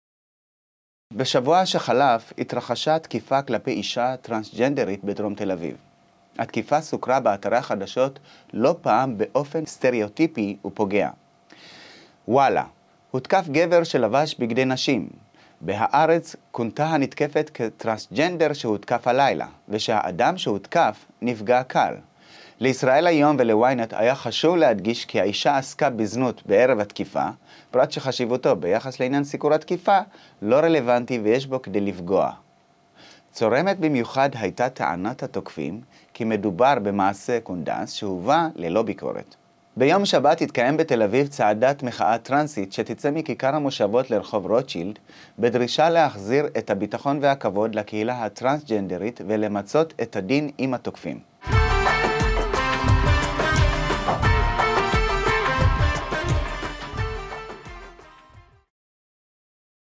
לוקחות את הלילה
בשבוע שחלף התרחשה תקיפה כלפי אישה טרנסג'נדרית בדרום תל אביב. התקיפה סוקרה באתרי החדשות לא פעם באופן סטריאוטיפי ופוגע. ביום שבת (11/1/2014) תתקיים בתל אביב  צעדת מחאה טרנסית, שתצא מכיכר המושבות לרחוב רוטשילד, בדרישה להחזיר את הביטחון והכבוד לקהילה הטרנסג'נדרית ולמצות את הדין עם התוקפים.